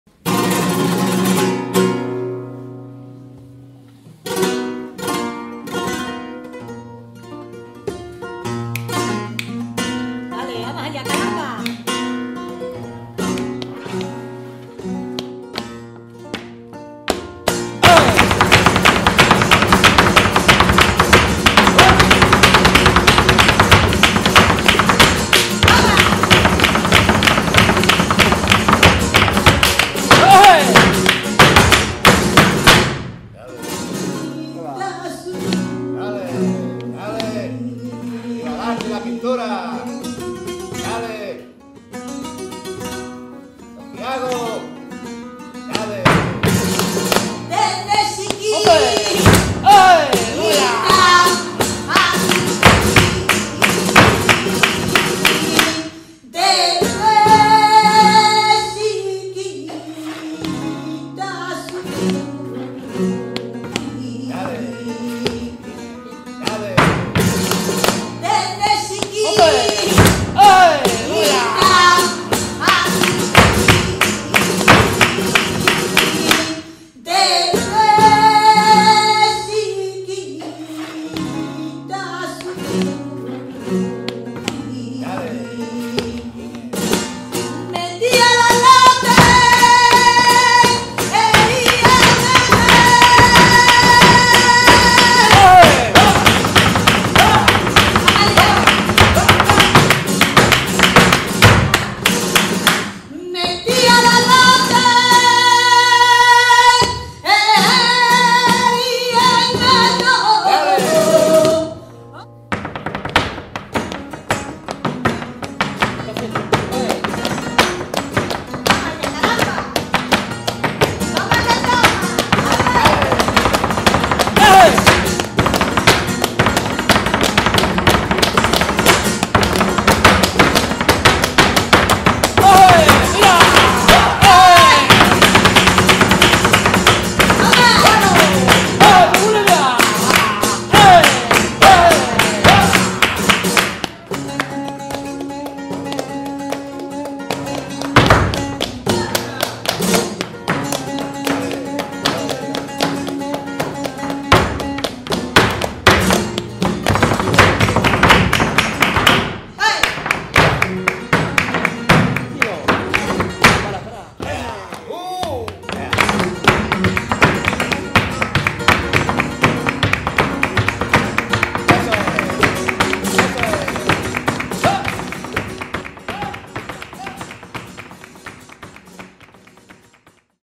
Pintura flamenca en vivo y en 3D
La artista alemana Sandra Hundelshausen inaugura una muestra de sus obras en el Centro Andaluz de Flamenco con una 'performance' en la que fluyen su pincel y el arte jondo en directo hasta confluir